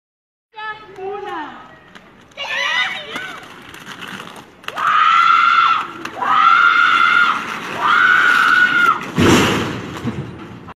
Girls Rollerblade Down A Ramp Into Gate Doors | Dumb People Doing Things Clips
The source for the best dumb people clips the internet has to offer.